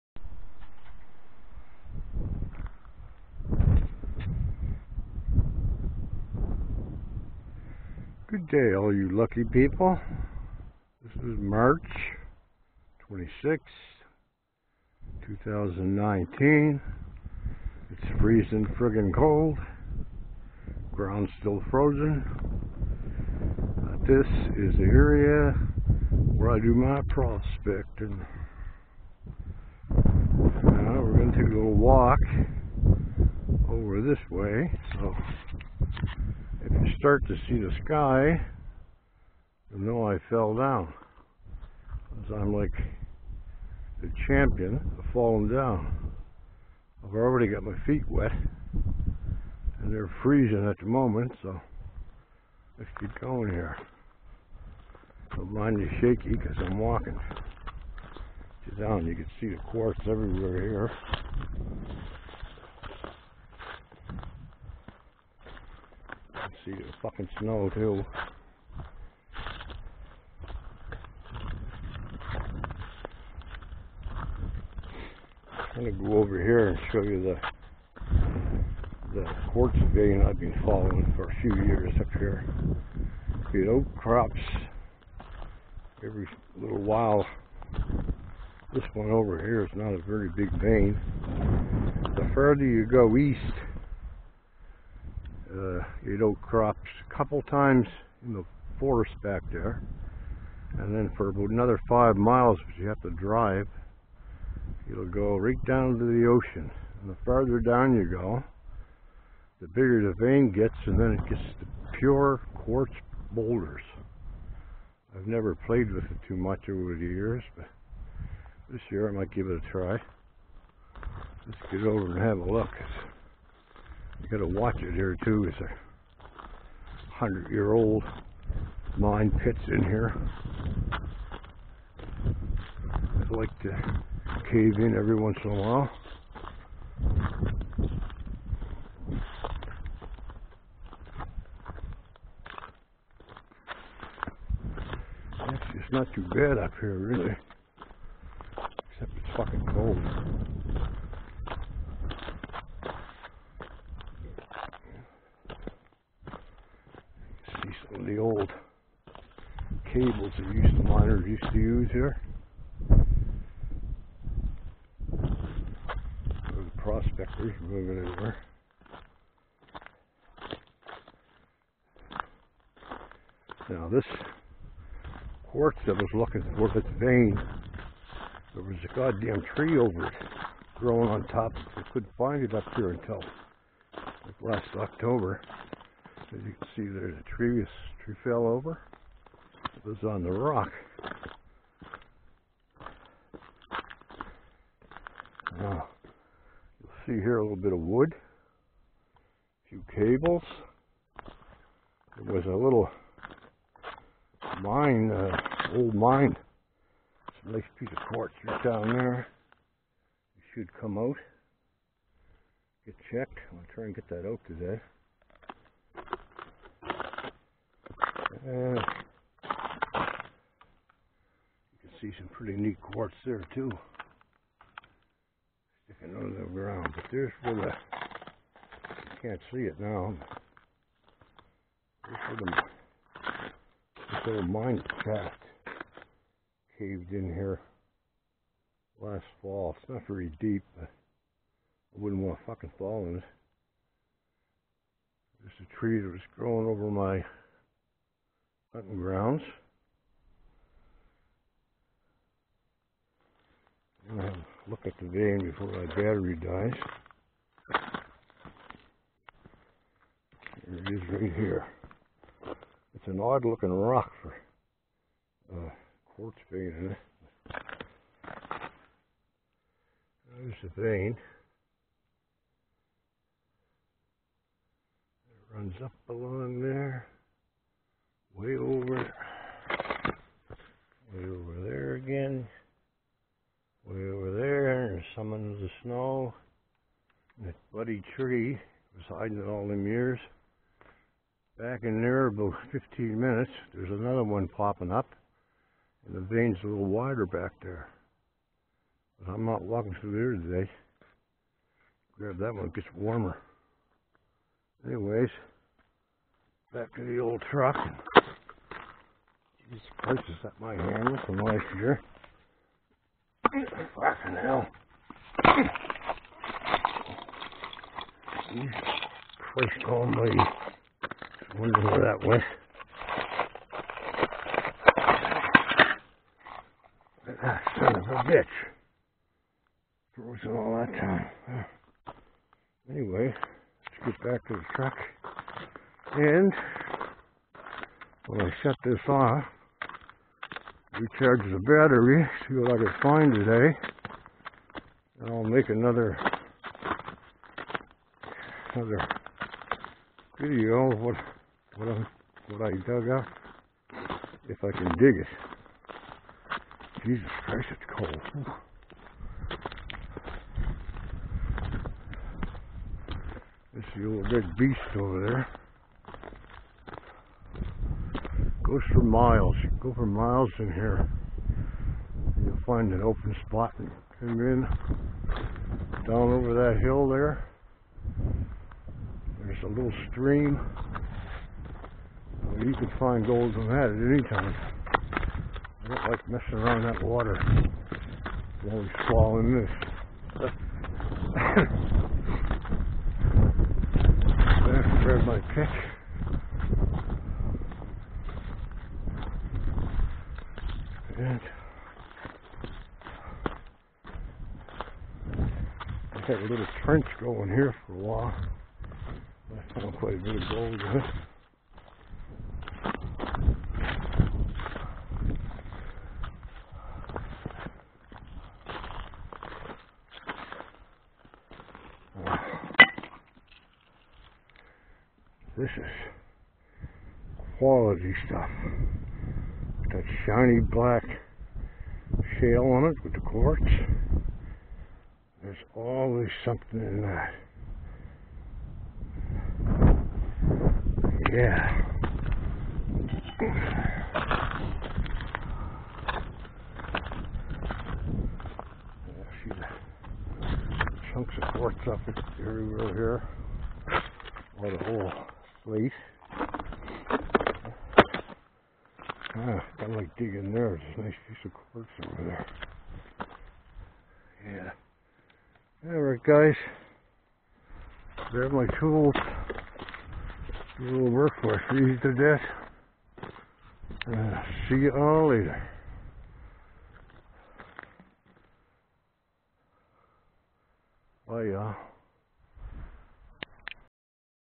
GOLD PROSPECTING MARCH 2019-COLD
FIRST TRIP THIS YEAR PROSPECTING, MAN IT WAS COLD 3 BELOW ZERO, FIRST THING, RUSSIAN ARMY BOOT SUCK, I STEPPED IN THE RIVER, INSTANT FULL OF WATER, MY FEET ARE FREEZING, GOT TO MY AREA, GROUND STILL FROZEN, CAN ONLY DIG 3 INCHES THEN SOLID ICE, SO I DUG AROUND THE DIRT, FROM OLD MINE TRENCHES, COME UP WITH GREAT SAMPLES, ORE ABOUT 7 OR 8 DIFFERENT TYPES WHICH I,LL CLEAN UP AND POST A FEW DAYS,  WELL I MANAGED TO FALL AGAIN, I COULD HAVE WON GOLD AT OLYMPICS FOR THE TWO TUMBLES, AND ROLLS, HAD TO HIT A TREE WITH MY FACE TO STOP..SO IT STILL A GOOD DAY, FEW MORE WEEKS IT WILL BE GOOD TO DIG,,THANKS
PS..I HAVE BEEN GOING HERE OVER 40 YRS, I HAVE NEVER CAME OUT WITH OUT A BRUISE , OR BLEEDING..BUT ITS FUN